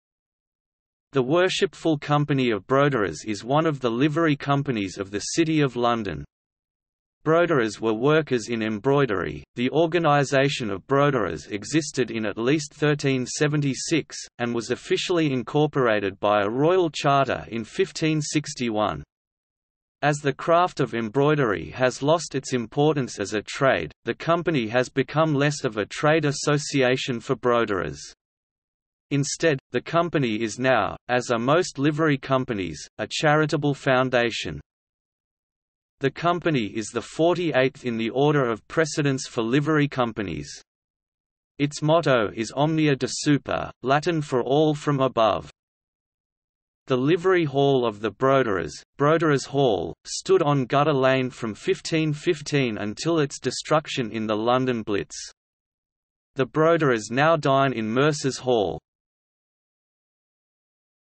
Worshipful Company of Broderers | Wikipedia audio article
- Socrates

SUMMARY
The Worshipful Company of Broderers is one of the Livery Companies of the City of London. Broderers were workers in embroidery; the organization of Broderers existed in at least 1376, and was officially incorporated by a Royal Charter in 1561. As the craft of embroidery has lost its importance as a trade, the Company has become less of a trade association for Broderers. Instead, the Company is now, as are most Livery Companies, a charitable foundation.
The Company is the forty-eighth in the order of precedence for Livery Companies. Its motto is Omnia Desuper, Latin for All From Above.
The livery hall of the Broderers, Broderers' Hall, stood on Gutter Lane from 1515 until its destruction in the London blitz. The Broderers now dine in Mercers' Hall.